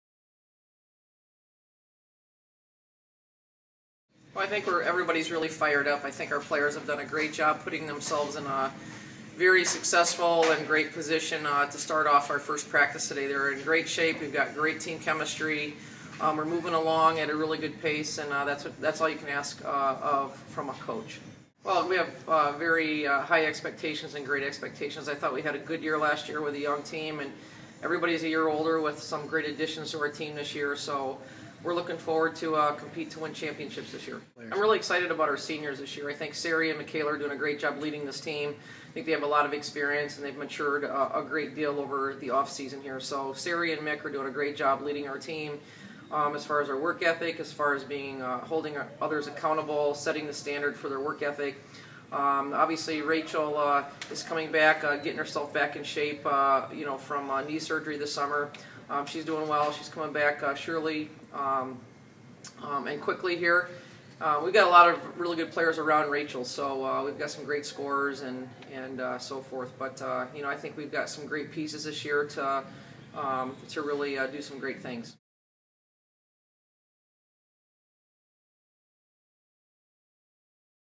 Pam Borton on Gophers' First Practice (Sept. 30, 2013)
Minnesota women's basketball coach Pam Borton talks about her team's first practice.